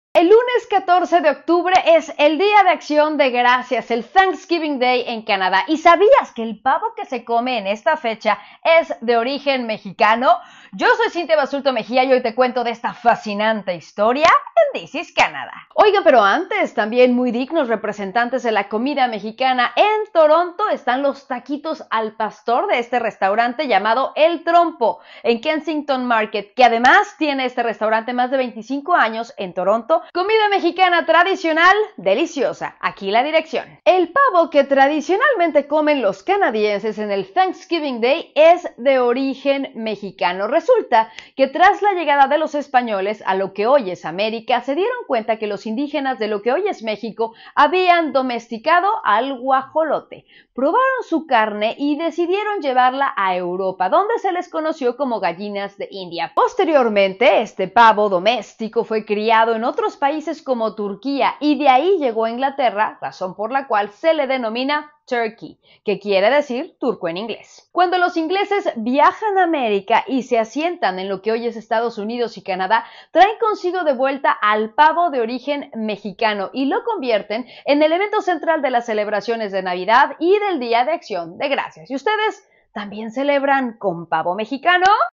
Origen del pavo de Thanksgiving day en Canadá
Conoce la historia del pavo de Thanksgiving. Este video llega a ti gracias a El Trompo.